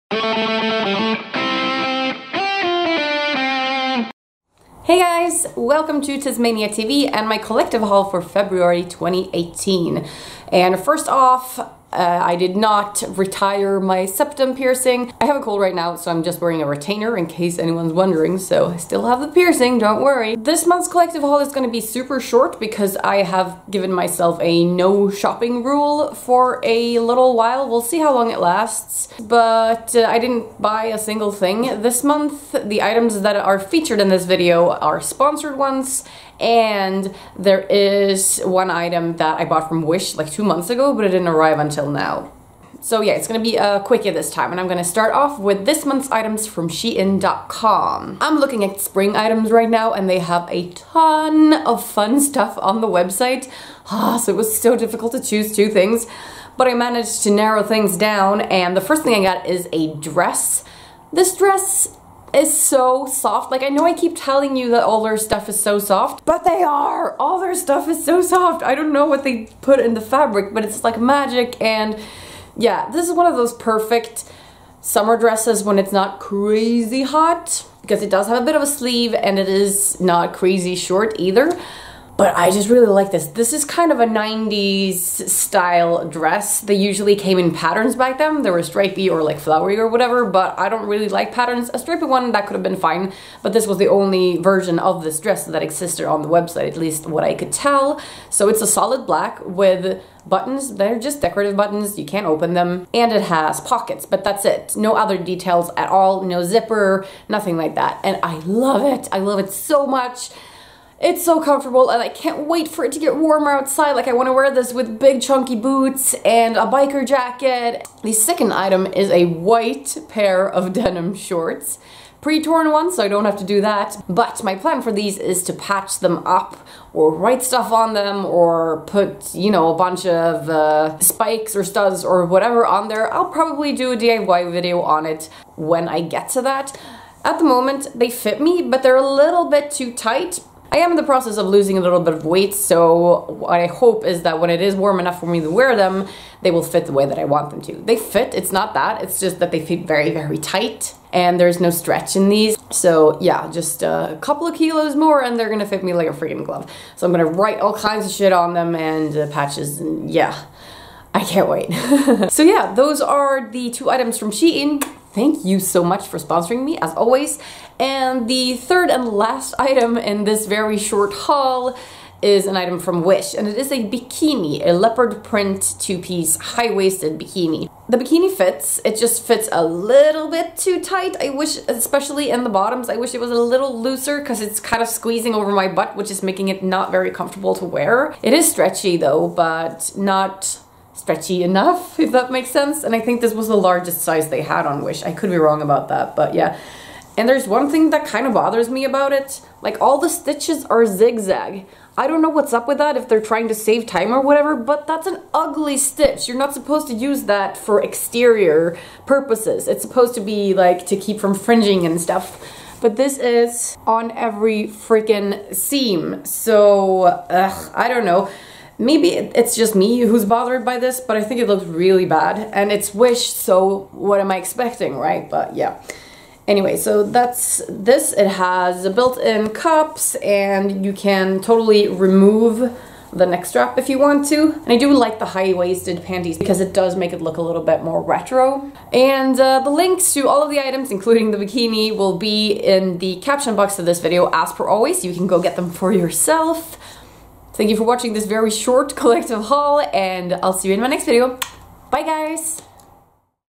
Collective Haul February 2018
Not a lot of stuff this month, so here's a quick haul for you guys!

VIDEO TIMELINE (direct links below)
00:44 Black dress & white shorts from Shein.com
02:42 Leopard print bikini from Wish

*Please note that I am located in Sweden, so all product features must allow for international shipping.